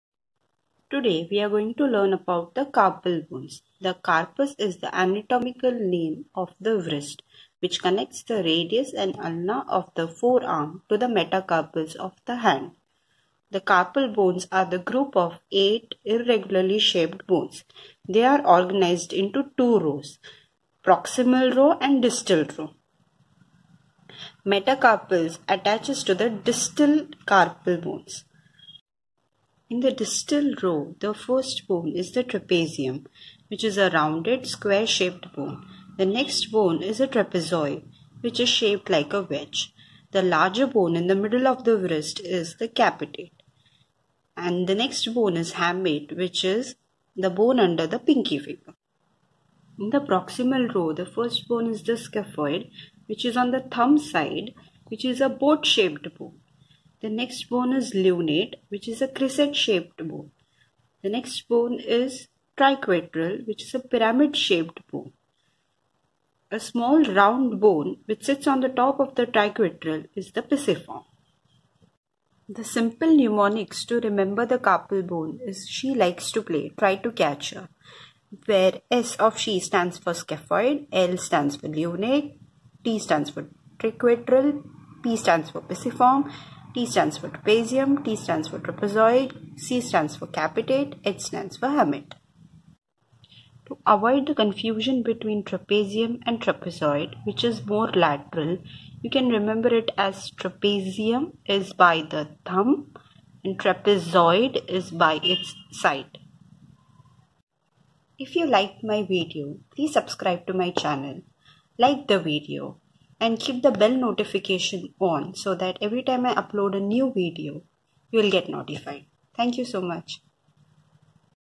Learn Anatomy of Carpal Bones with easy to remember Mnemonics
There are 8. no of carpal bones of wrist which attaches the bone of forearm to hand.
In this video I have shared tricks to learn the anatomy of carpal bones with simple mnemonics making it easy to remember.